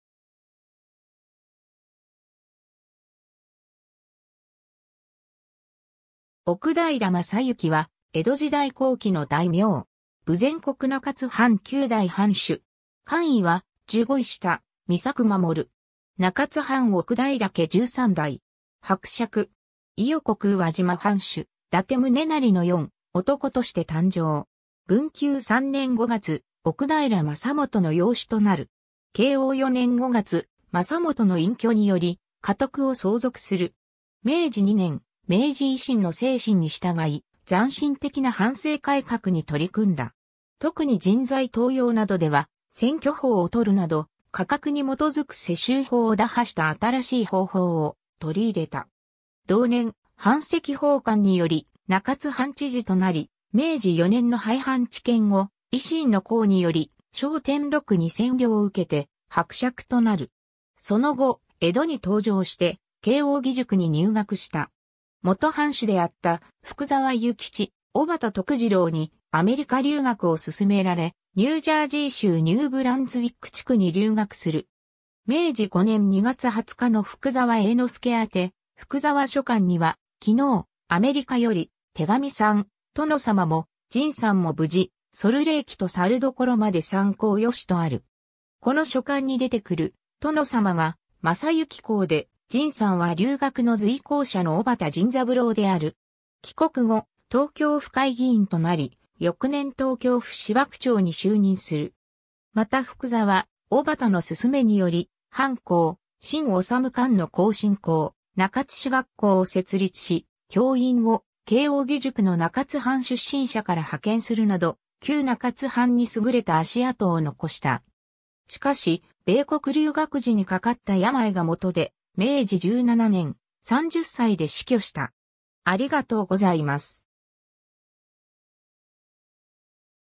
奥平昌邁
奥平 昌邁（おくだいら まさゆき）は、江戸時代後期の大名。
豊前国中津藩9代（最後の）藩主。
官位は従五位下・美作守。
中津藩奥平家13代。
伯爵。
 伊予国宇和島藩主・伊達宗城の四男として誕生。
文久3年（1863年）5月、奥平昌服の養子となる。
慶応4年（1868年）5月、昌服の隠居により家督を相続する。
明治2年（1869年）、明治維新の精神に従い、斬新的な藩政改革に取り組んだ。
特に人材登用などでは選挙法を採るなど、家格に基づく世襲法を打破した新しい方法を取り入れた。
同年、版籍奉還により中津藩知事となり、明治4年（1871年）の廃藩置県後、維新の功により賞典禄2000両を受けて伯爵となる。
 その後、江戸に東上して慶應義塾に入学した。
元藩士であった福澤諭吉、小幡篤次郎にアメリカ留学を勧められ、ニュージャージー州ニューブランズウィック地区に留学する。
明治5年（1872年）2月20日の福澤英之助宛福澤書翰には、「昨日アメリカより手紙参、殿様も甚さんもぶじ、ソルレイキと申処迄参候よし」とある。
この書翰に出てくる「殿様」が昌邁公で、「甚さん」は留学の随行者の小幡甚三郎である。
 帰国後、東京府会議員となり、翌年東京府芝区長に就任する。
また福澤、小幡の勧めにより藩校・進脩館の後身校「中津市学校」を設立し、教員を慶應義塾の中津藩出身者から派遣するなど、旧中津藩に優れた足跡を残した。
しかし、米国留学時にかかった病が元で、明治17年（1884年）、30歳で死去した。